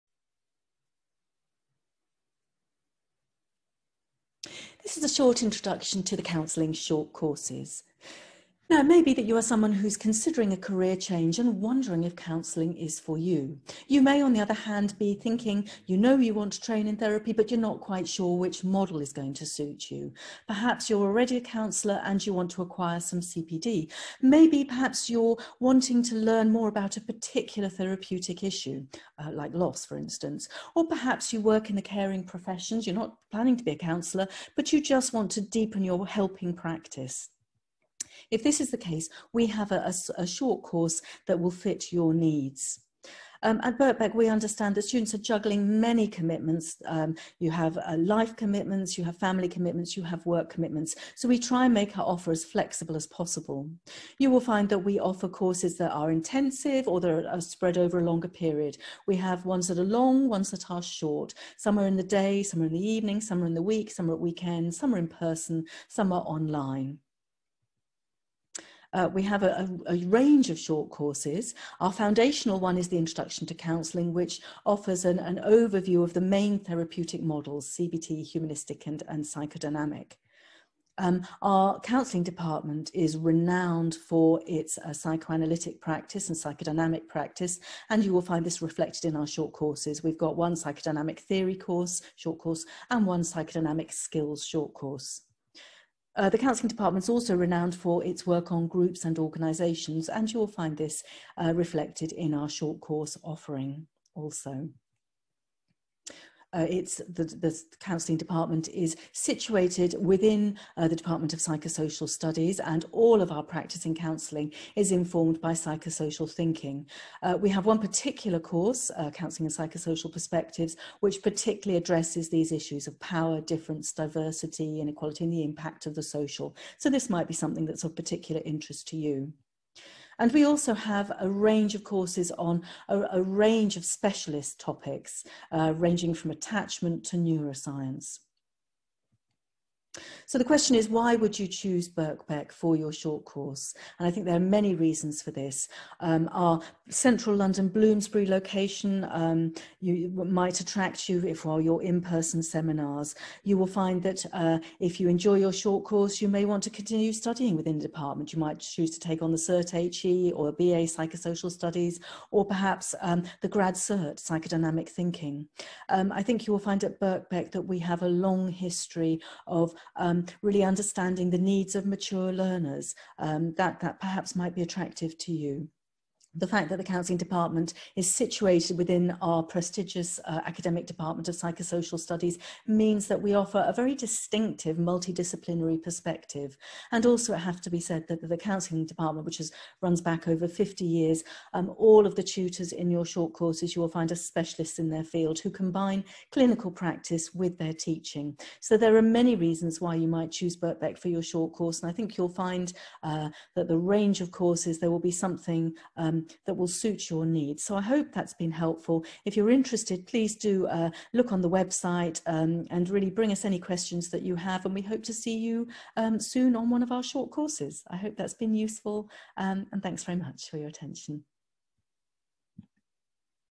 Counselling short courses - an introduction
Looking to find out more about Counselling? This video will provide an introduction to the Counselling short courses we offer at Birkbeck, University of London, provided by our Department of Psychosocial Studies.